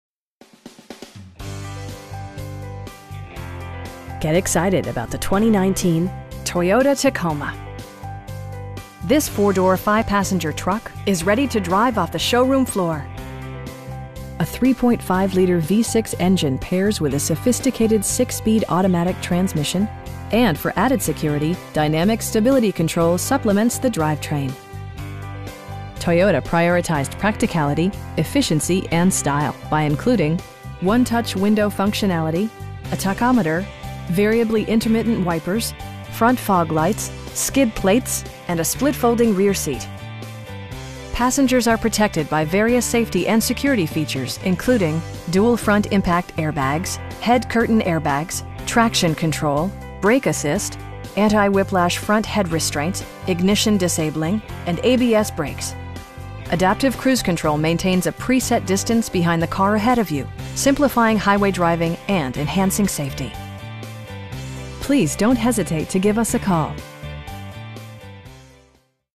2019 Toyota Tacoma TRD Sport V6 in Conroe, TX 77304
Get excited about the 2019 Toyota Tacoma. This 4 door, 5 passenger truck is ready to drive off the showroom floor! A 3.5 liter V-6 engine pairs with a sophisticated 6 speed automatic transmission, and for added security, dynamic Stability Control supplements the drivetrain. Toyota prioritized practicality, efficiency, and style by including: 1-touch window functionality, a tachometer, variably intermittent wipers, front fog lights, tilt and telescoping steering wheel, skid plates, and a split folding rear seat. Passengers are protected by various safety and security features, including: dual front impact airbags, head curtain airbags, traction control, brake assist, anti-whiplash front head restraints, ignition disabling, and ABS brakes. Adaptive cruise control maintains a pre set distance behind the car ahead of you, simplifying highway driving and enhancing safety. We have the vehicle you've been searching for at a price you can afford. Please don't hesitate to give us a call.